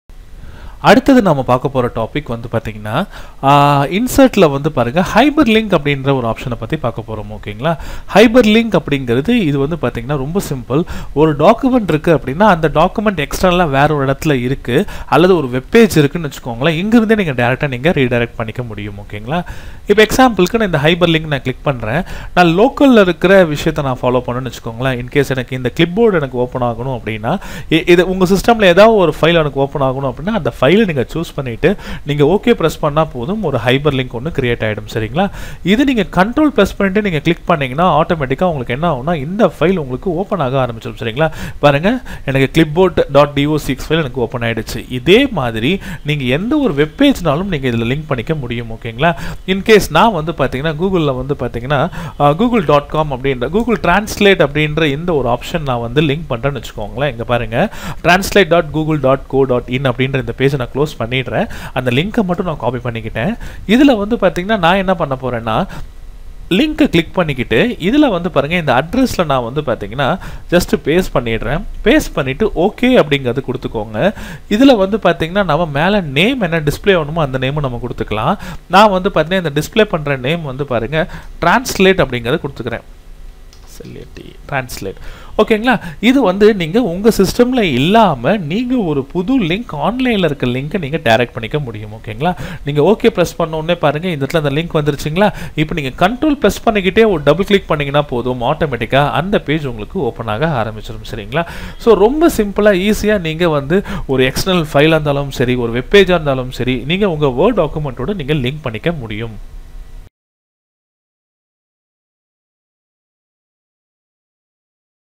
HyperLink in Microsoft Word Tamil | Insert Menu | COA | TNTCIA |TNDTE
How to use SmartArt in Microsoft Word

Computer on Office Automation Training Salem
What are the Theory & Practical for Computer on Office Automation?